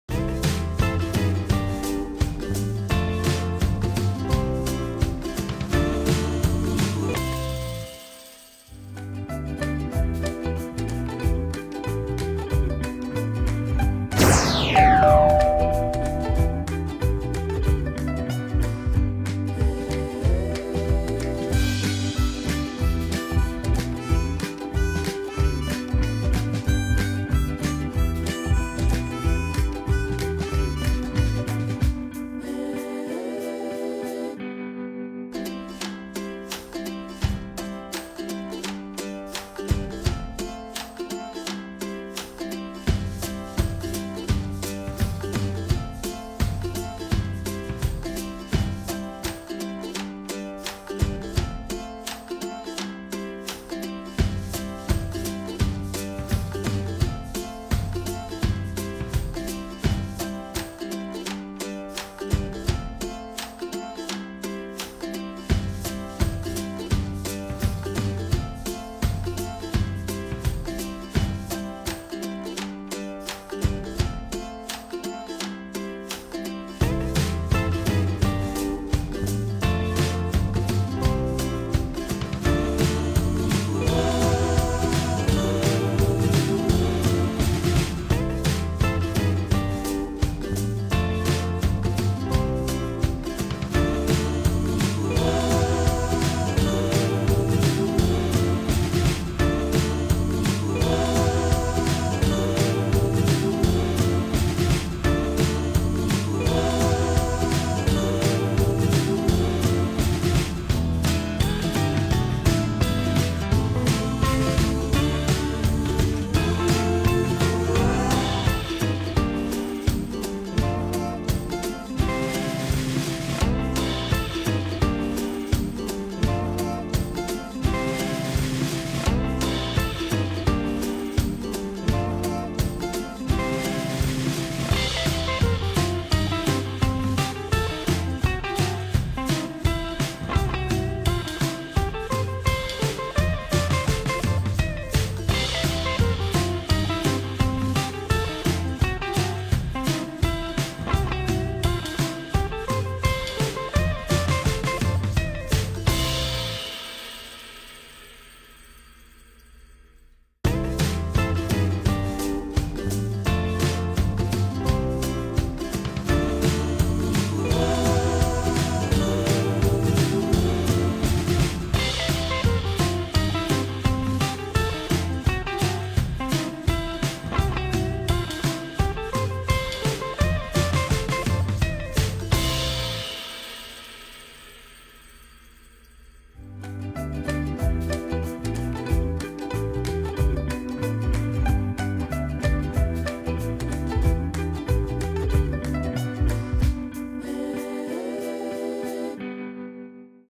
Fitness Photoshoot With My Friend - Hardwork Pays Off-
Last Sunday my friend and I had a Fitness Photoshoot in our Gym. My husband took the pictures, as you all may know he is a professional photographer and can take beautiful pictures. Our photoshoot was a blast, it was a lot of posing but definitely worth it .

Camera: iPhone
Video Editing: iMovie
Music From: iMovie
Intro & Font: Phonto